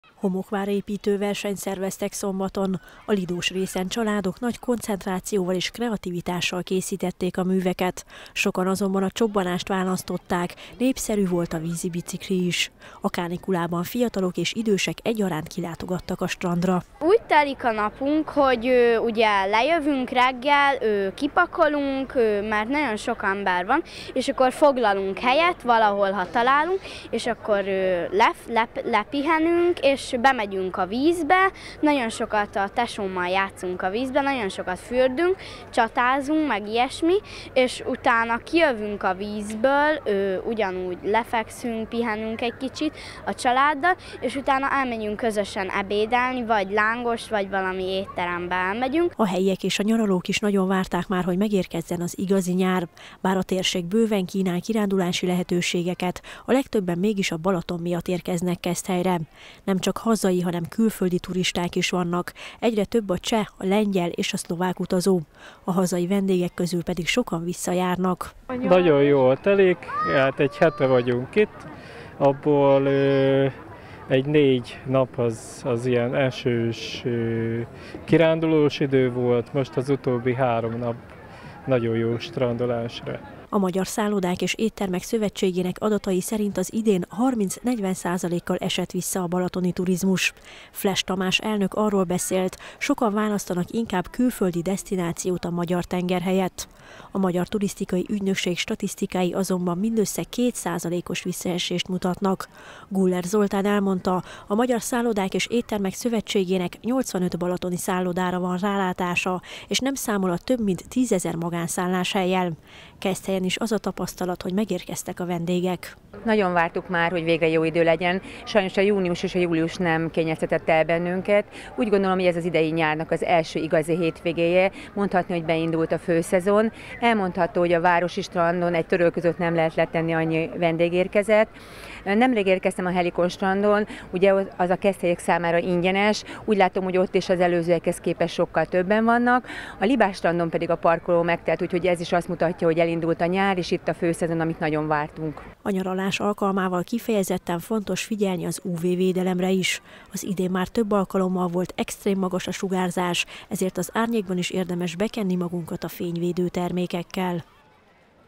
Megjött a nyár
Megteltek a strandok a hétvégén. Az idei főszezonban ez volt az első olyan alkalom, amikor igazi nyári hőség volt. Keszthely három fürdőhelye népszerű a turisták körében, sokan már évek óta visszajáró vendégek. A Városi Strandon a fürdés és a napozás mellett programokat is kínálnak a vendégeknek.